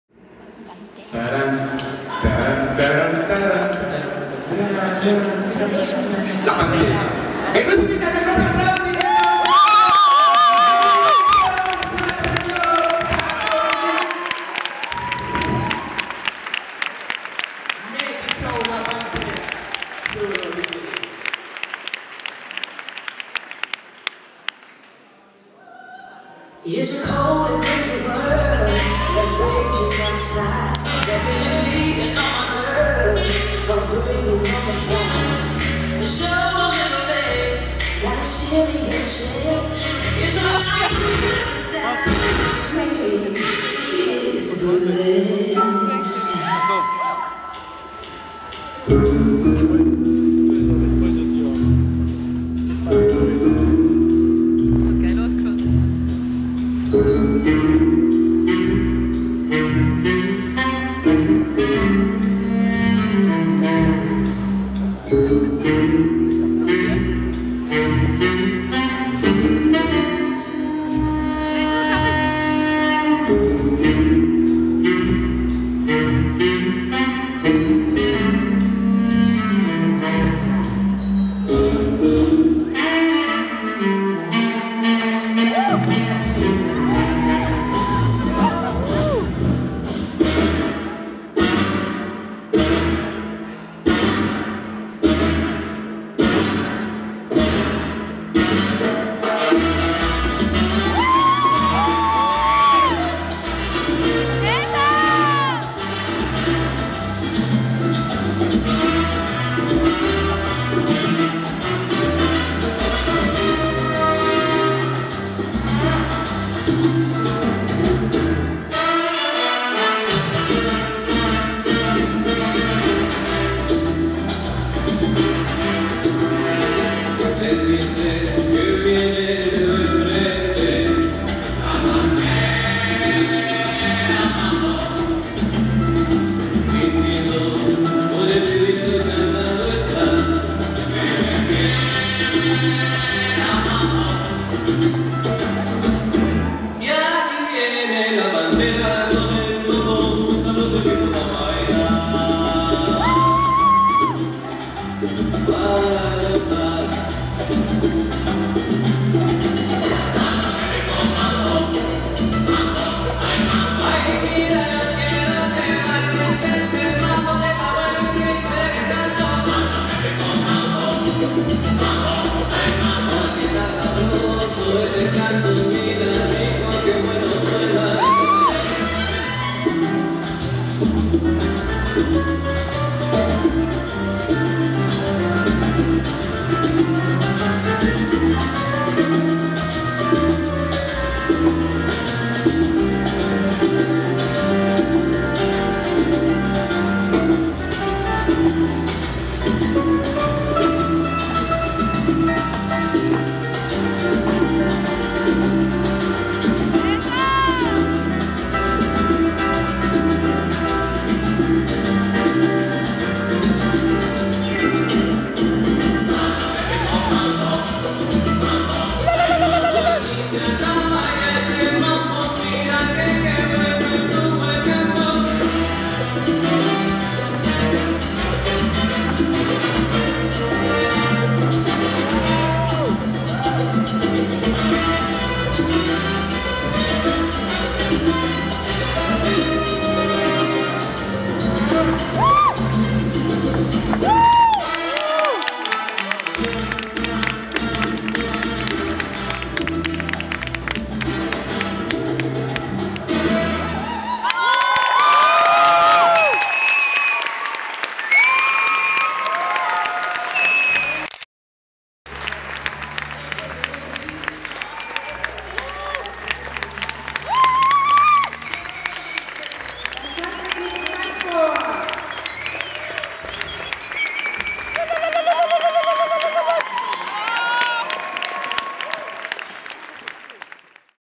La Pantera (Hall in Tirol 13.01.2012) von der Show Gruppe des Salsa Club Salzburg
Dies ist eine weitere Aufzeichnung von unserer Show beim 9. Salsa Congress Austria in Hall in Tirol.

Die Choreographie der LA Show ist im Teamwork zwischen Edith HOMOKI, Marilis STERLINGER, Patrick STEINDL